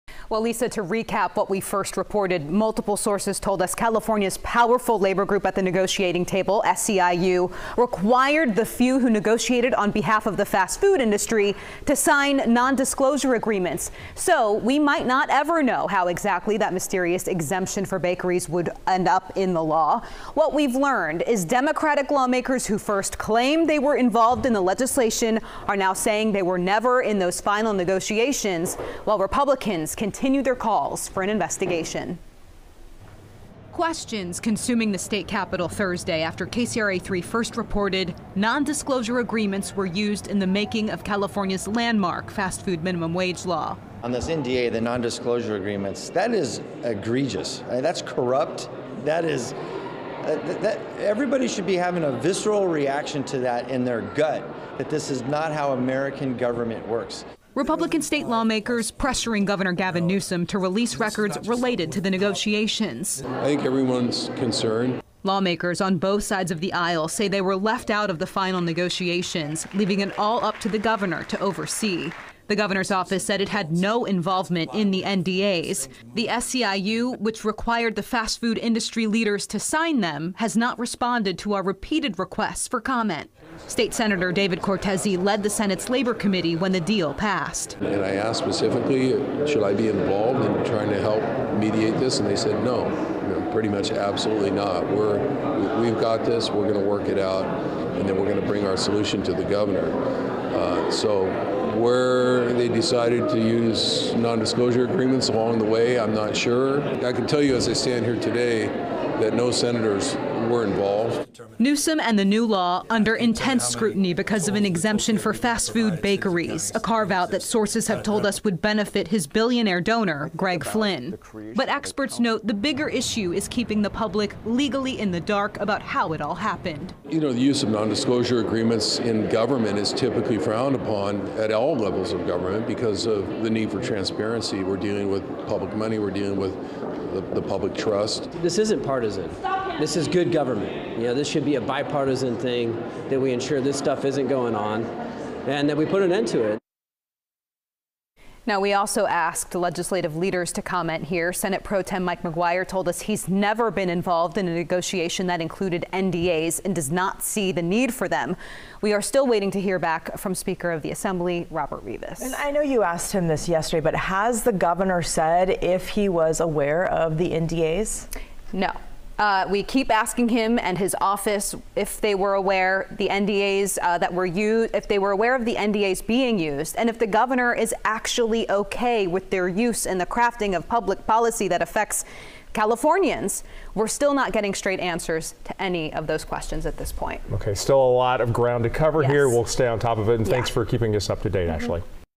California lawmakers react to use of NDAs in fast food minimum wage law
KCRA 3 follows up with lawmakers as California's new fast food minimum wage law comes under fire.